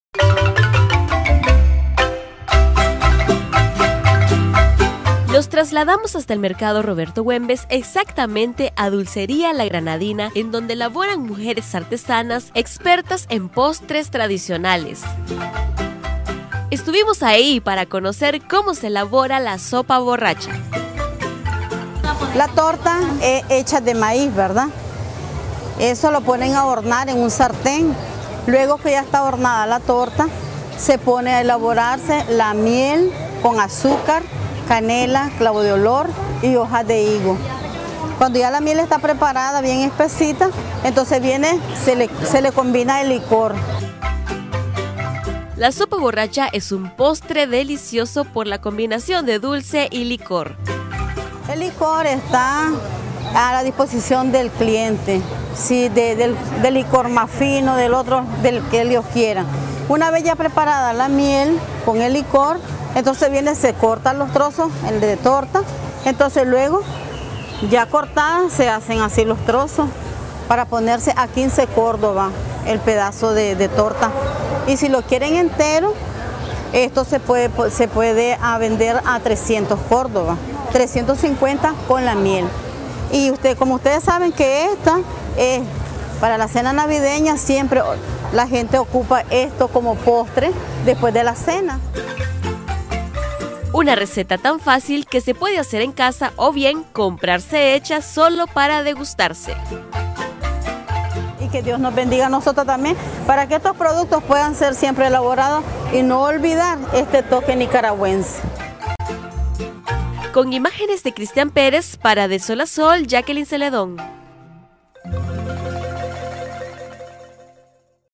Receta de la Sopa Borracha, postre navideño nicaragüense
El mes de diciembre es aprovechado por muchos comerciantes del Mercado Roberto Huembes para vender diversos productos alusivos a la Navidad.